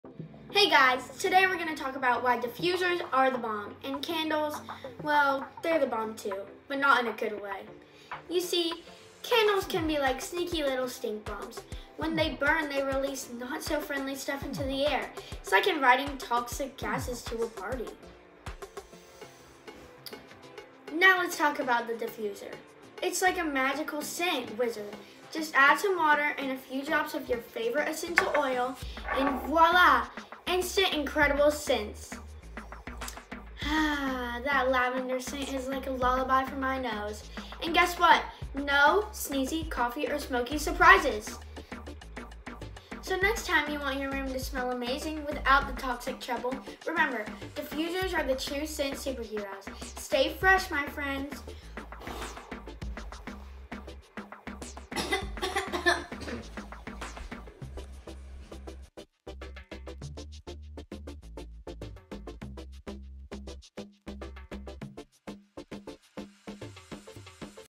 Candles vs. Diffusers
Candles vs. Diffusers - The Choice is Clear!

🕯️ Candles have been a go-to for ambiance, but let’s talk about the hidden downside - TOXICITY! 😱
🚫 Many candles emit harmful chemicals when burned, like lead, benzene, and formaldehyde. 😷 Not exactly what you want in your home, right?

🌿 Diffusers use essential oils, nature’s fragrances, without the harmful toxins. 🌱 Safe, clean, and oh-so-soothing! 😌

Make the switch for a healthier, happier home! 🏡💚 DiffusersForLife CleanLiving ToxicFreeLiving 🌿🕯️